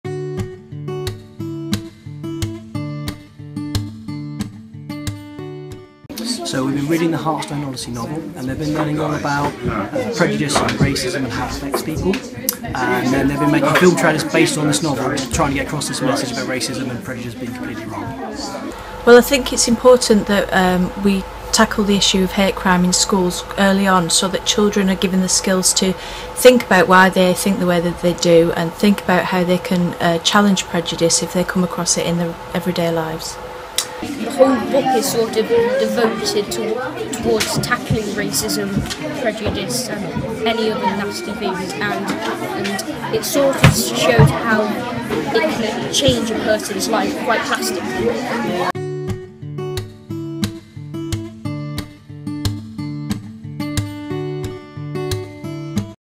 Heartstone Project in Schools
Ten Nottinghamshire schools are taking part in Heartstone workshops tackling prejudice and discrimination which are part funded by Nottinghamshire County Council.

All Saints Anglican and Methodist Aided Primary School in Elston is one of these schools and has been producing film trailers that express the Heartstone themes.

Transcript

John Powell (Teacher Elston All Saints Primary)

"So we've been reading the Heartstone Odyssey novel and they've been learning all about prejudice and racism and how it affects people. And then they will make film trailers based on this novel to try to get across this message about racism and prejudice being completely wrong."

Councillor Alice Grice (Nottinghamshire County Council)

"Well I think it's important that we tackle the issue of hate crime in schools early on so that children are given the skills to think about why they think the way that they do and think about how they can challenge prejudice if they come across it in their everyday lives."

Archie and Toby (Students)

Toby

"The whole book is sort of devoted towards tackling racism and prejudice and many other nasty things and it sort of shows how it can change a person's life quite drastically."